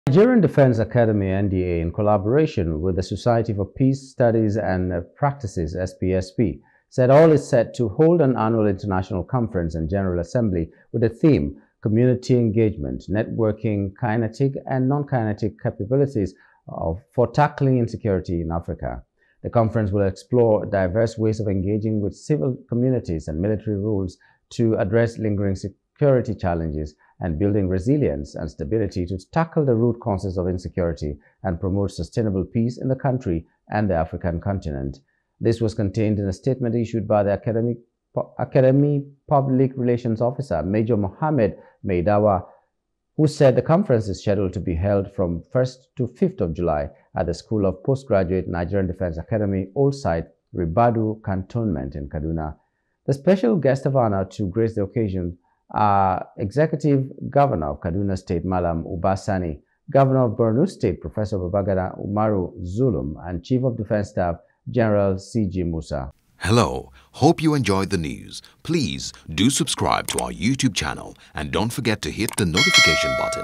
NDA Holds Conference On Networking Kinetic And Non-Kinetic Capabilities For Insecurity In Africa
The Nigerian Defence Academy (NDA) in collaboration with the Society for Peace Studies and Practice (SPSP) said all is set to hold an Annual International Conference and General Assembly with the “theme Community Engagement: Networking Kinetic and Non-Kinetic Capabilities for Tackling Insecurity in Africa.”
The conference will explore diverse ways of engaging with civil communities and military roles to address lingering security challenges build resilience and stability to tackle the root causes of insecurity and Promote sustainable peace in the country and the African continent.